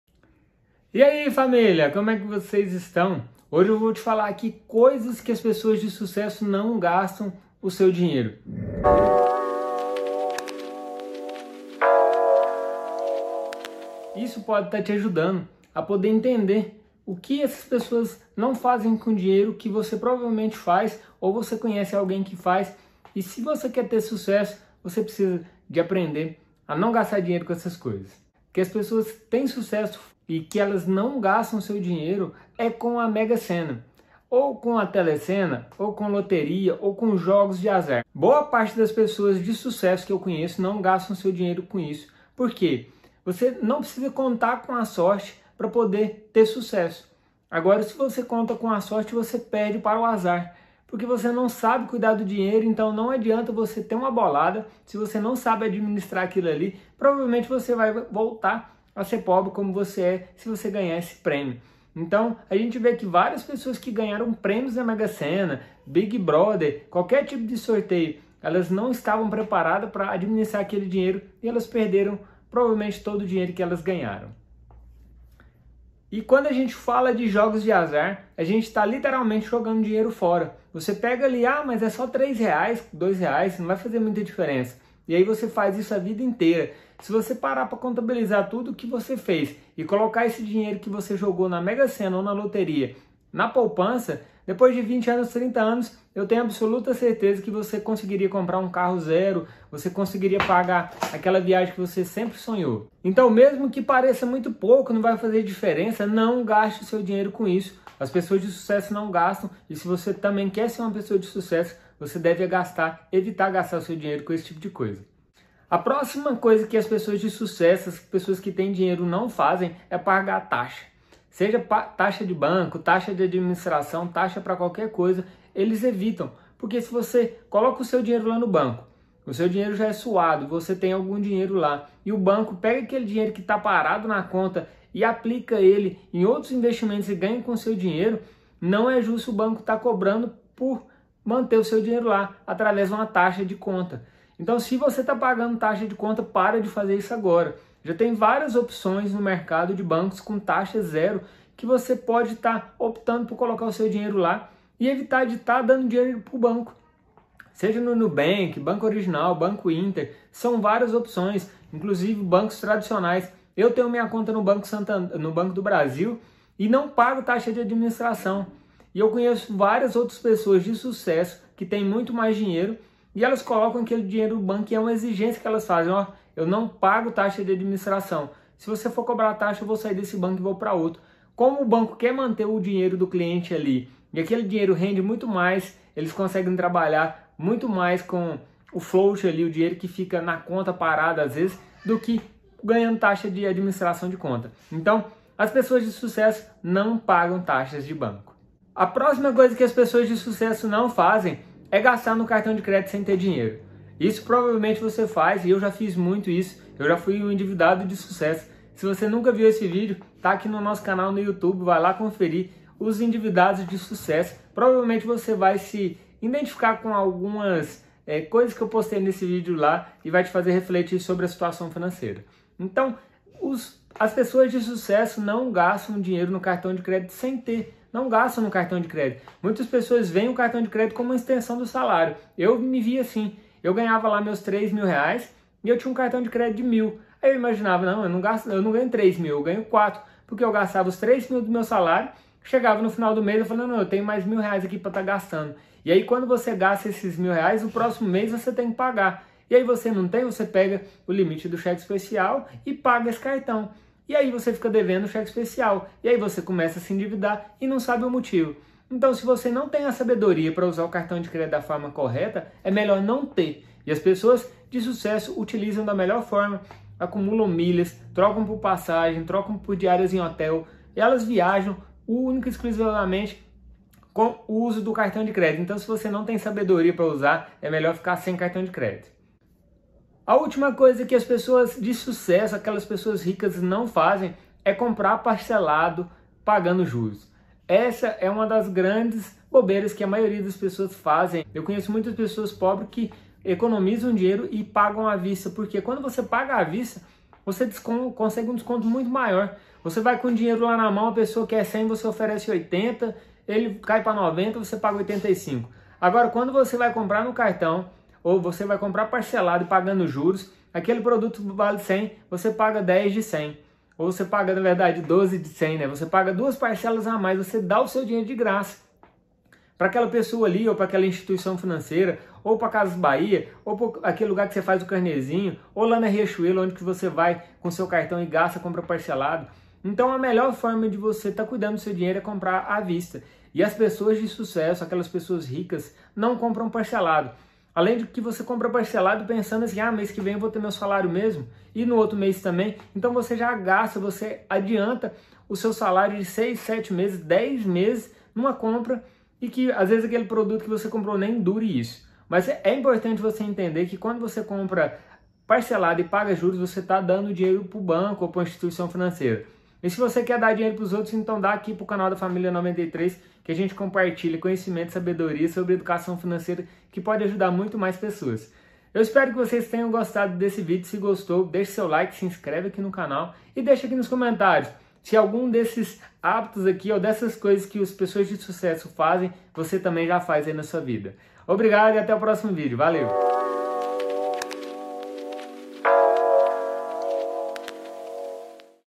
COISAS QUE AS PESSOAS DE SUCESSO NÃO FAZEM COM SEU DINHEIRO - EDUCAÇÃO FINANCEIRA
Nos períodos de crise econômica é preciso apertar o cinto e o orçamento, já que os impactos são sentidos diretamente no nosso bolso. Saiba o que não fazer com seu dinheiro e o que evitar durante esta época complicada!
Não perca mais tempo para cuidar da sua vida financeira.